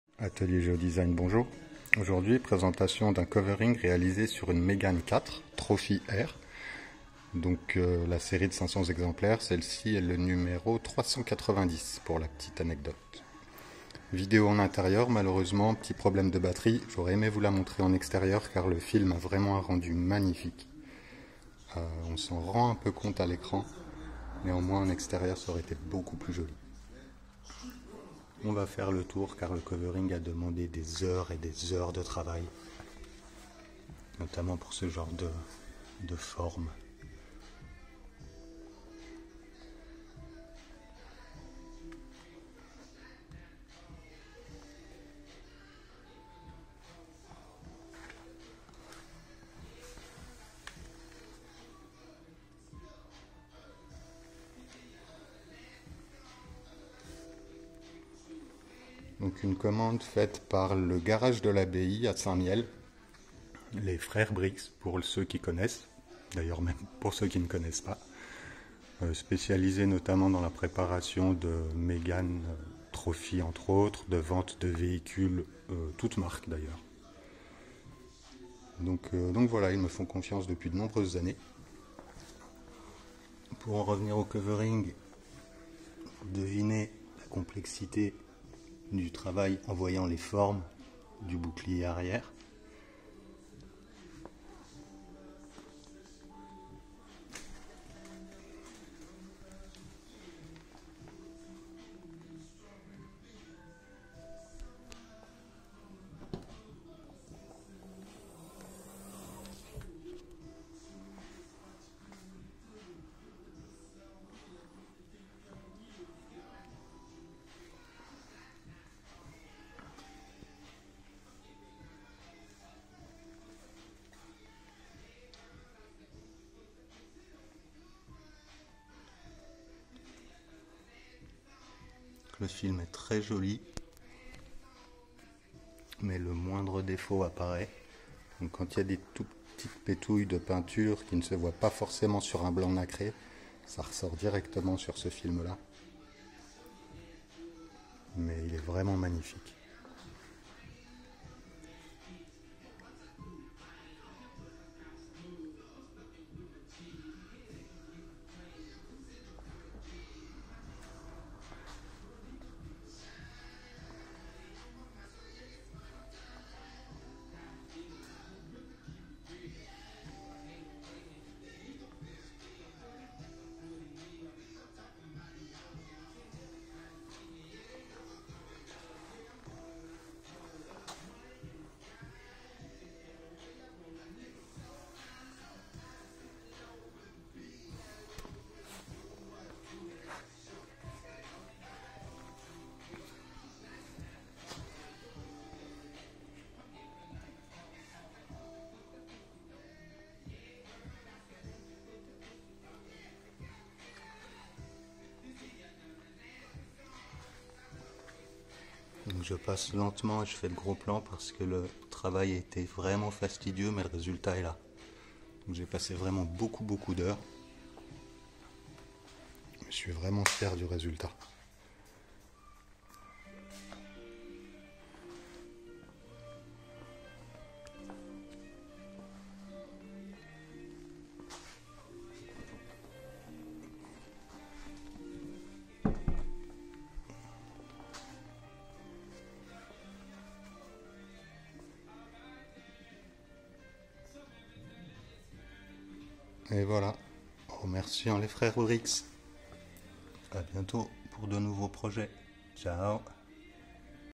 Mégane 4 Trophy-R covering complet par l'atelier Géo-Design.
Covering complet jaune satin métallisé sur une Megane 4 Trophy-R  du Garage Brix à Saint Mihel.